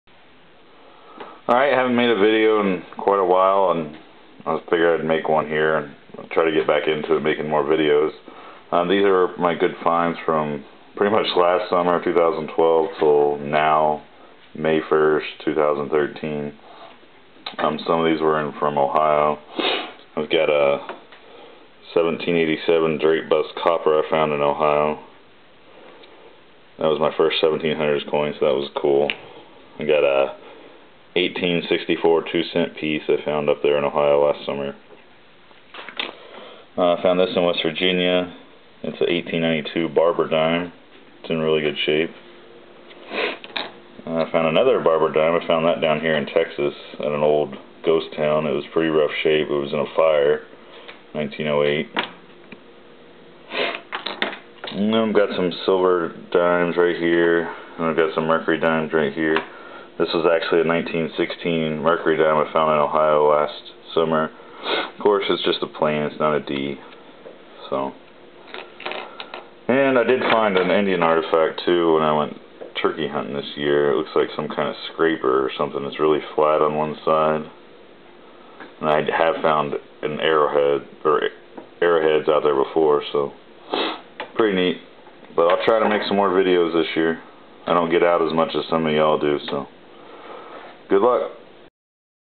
Metal Detecting Finds Summer 2012-Spring 2013
Finds from metal detecting summer 2012-spring 2013 and include coins from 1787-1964 from Ohio, West Virginia, and Texas.